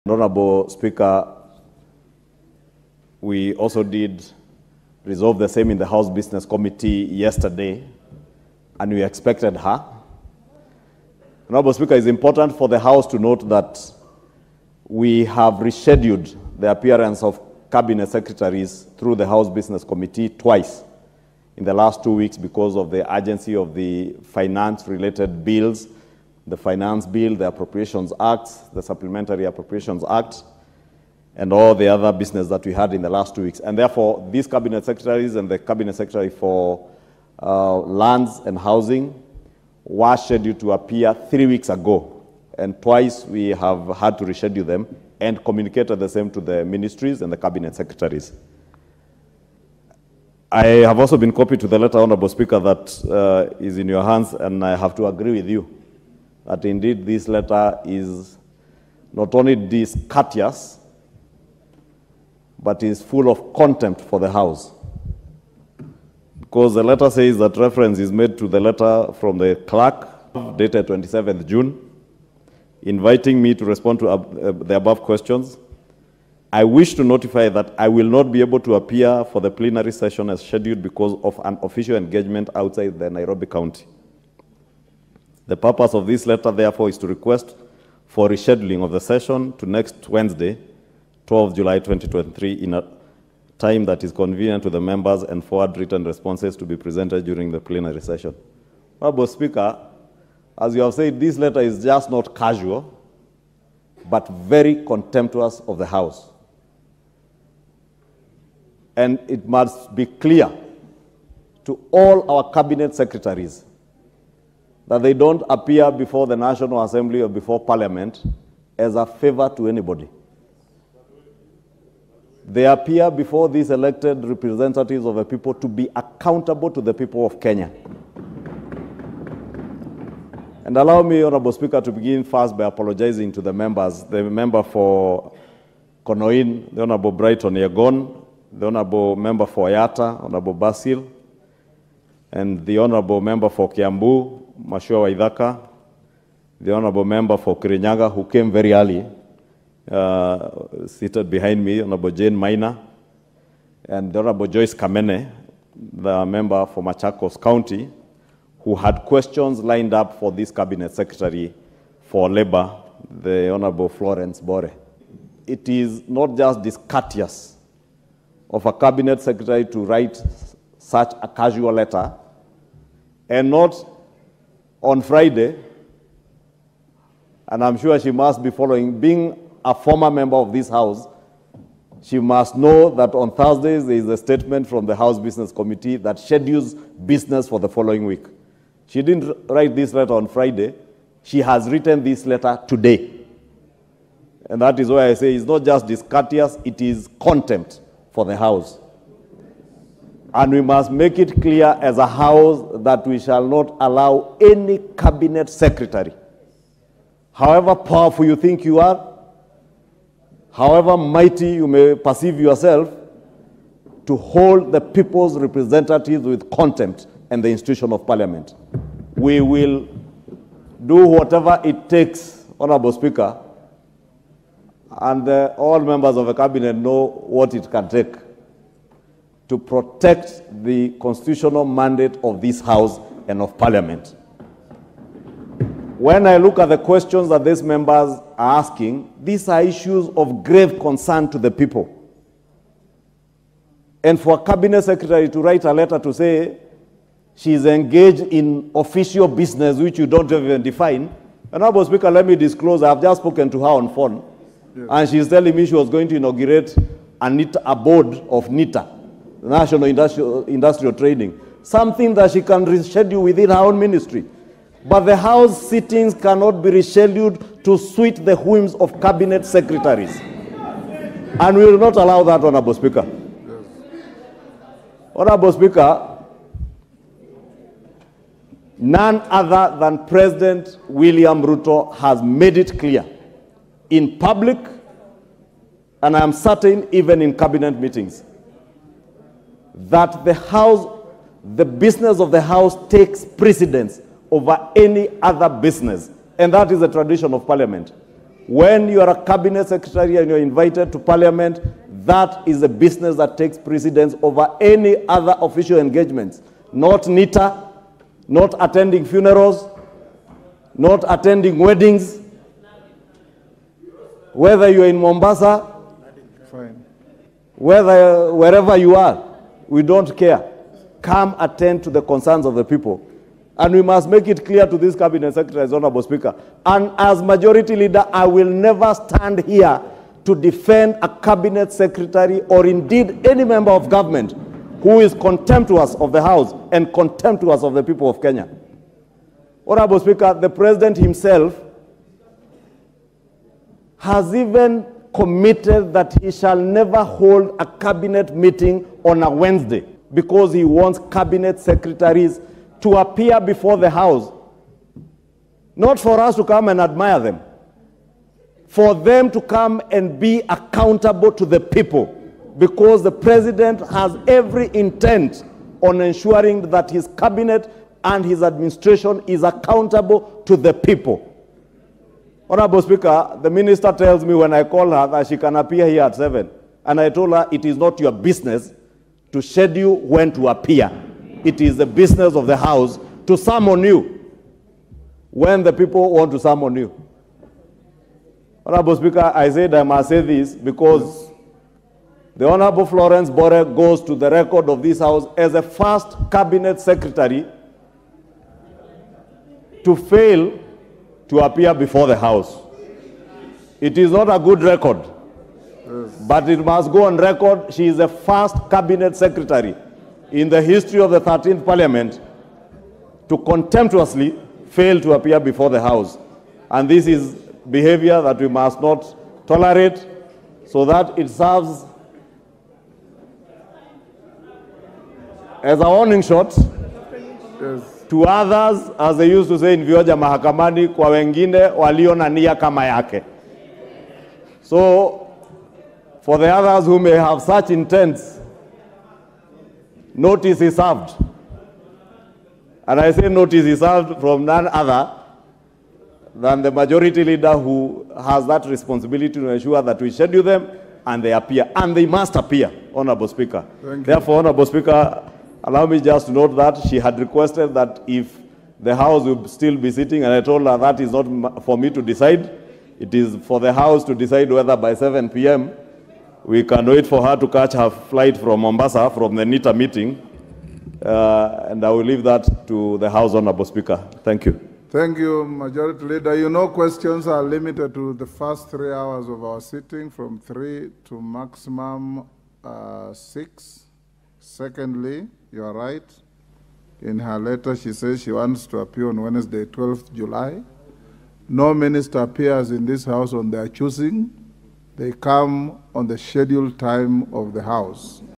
CRISIS IN KENYA KWANZA AS ICHUNGWA & WETANGULA LECTURE RUTO'S CS FLORENCE BORE IN PARLIAMENT
Kenya News Channel brings you the Latest News Today. Get more at Citizen TV Live Streaming Kenya Now, NTV Kenya Livestream and KTN News Live.